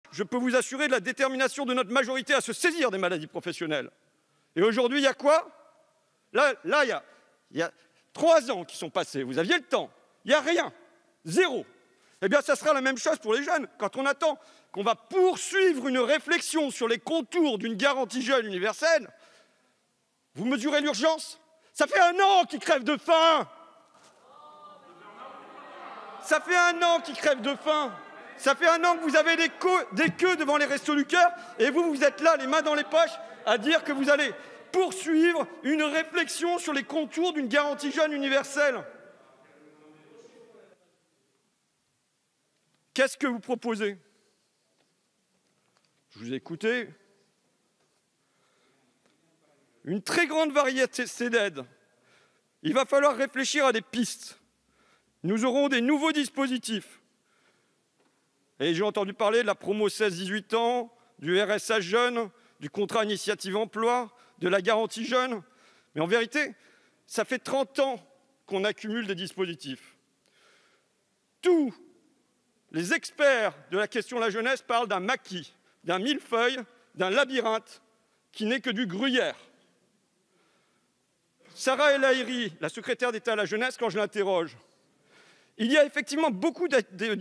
«Ça fait 1 an qu'ils crèvent de faim» : la grosse colère de François Ruffin à l'Assemblée
Dans le cadre de sa niche parlementaire qui a lieu ce 6 mai 2021, le groupe parlementaire de La France insoumise (LFI) défend l’extension du RSA dès 18 ans. Quand on a entre 18 et 25 ans en France aujourd’hui, que l’on a pas le soutien financier de ses parents, on est abandonné par la République. Les images d’étudiants faisant des files d’attentes interminables pour pouvoir manger sont là pour en témoigner.

La proposition de loi des insoumis, présentée à l’insoumission par son rapporteur le député de la Somme François Ruffin, est plébiscitée par une majorité de Français. Une étude de l’institut de sondage Harris interactive, publiée ce 5 mai 2021, révèle en effet que près des deux tiers des Français (63%) sont favorables à l’extension du RSA aux 18/25 ans.

Ce 6 mai 2021, François Ruffin a fait résonner les murs de l'hémicycle de l'Assemblée nationale : «Ça fait 1 an qu'ils crèvent de faim ! Ça fait 1 qu'ils crèvent de faim ! Ça fait 1 an les files d'attentes interminables devant le Resto du cœur ! Et vous êtes les mains dans les poches.» À bon entendeur...